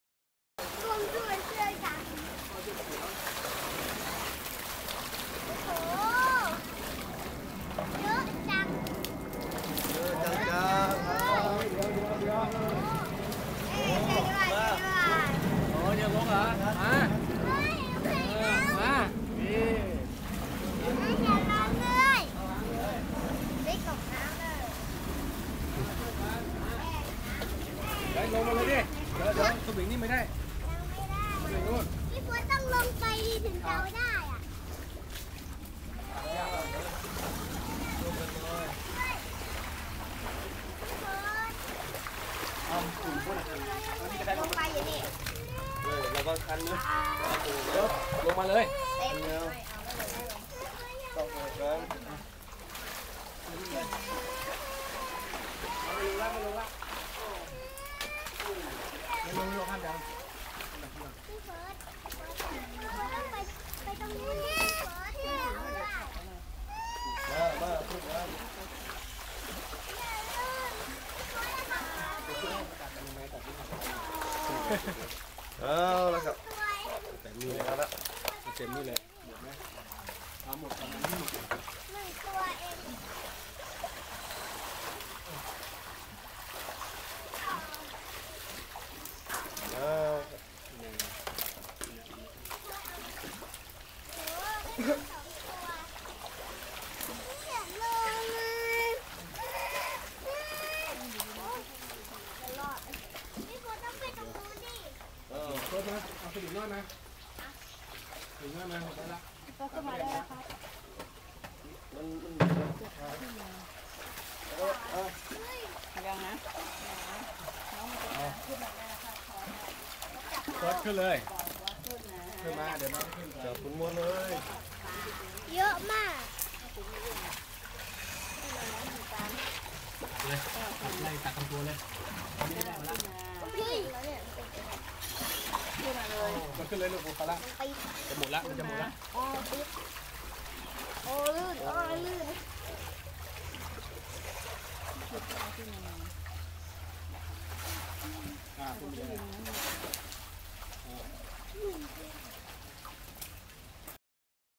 บ่อมานพ ตักปลาปล่อยรอบบ่าย แมท 10,000 อาทิตย์ 18/10/2558 (EP.1/2)
ตักปลาปล่อยรอบบ่าย แมท 10,000 อาทิตย์ 18/10/2558

พบกันอีกครั้ง ทุกวันอาทิตย์ กับการแข่งขันตกปลาดุก ชิงเงินรางวัลเงินสด 10,000 บาท
และ รางวัลอื่นๆ อีกมามาย ณ.บ่อตกปลามานพ เชียงใหม่...ห้ามพลาด!